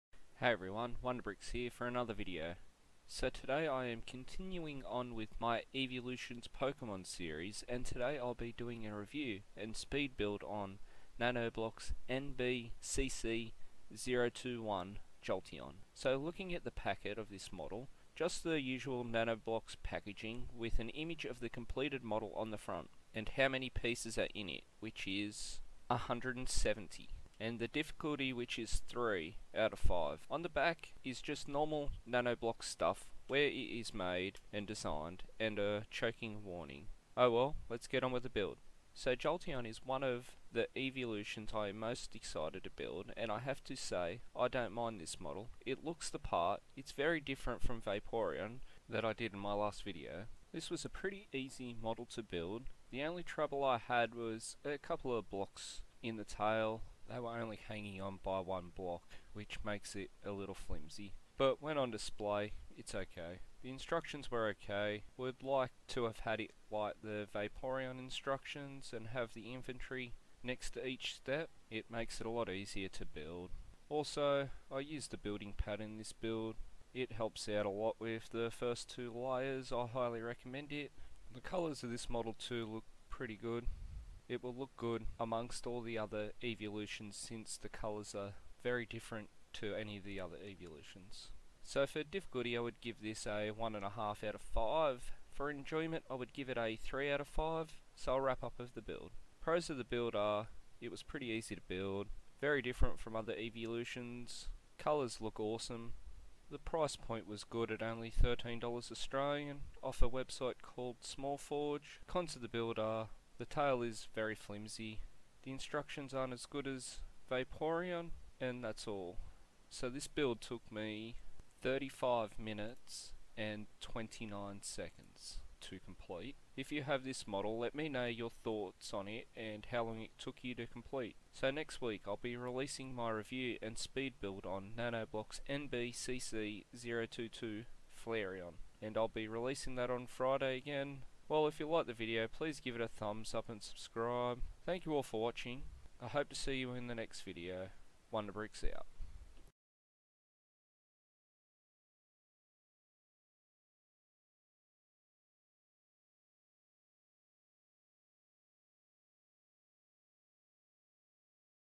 NANOBLOCK POKEMON ( JOLTEON ) REVIEW AND SPEED BUILD
In this video I am doing a review and speed build on NBPM 021 JOLTEON

in this set there is 170 pcs

this build took me 35 mins and 29 seconds to complete